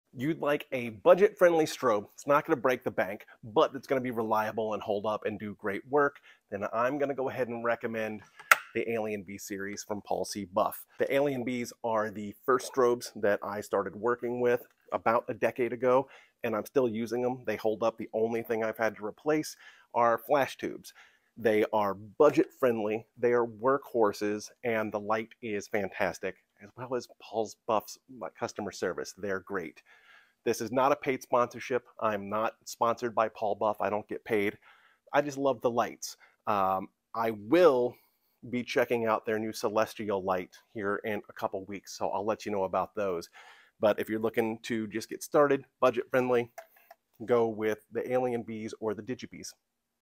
The @PaulcbuffInc Alien Bee series is a fantastic, budget friendly strobe, and is a work horse
Ive been working with the @paulcbuffinc alien Bee 800 strobes since i started working with stobes. They are absolute work horses.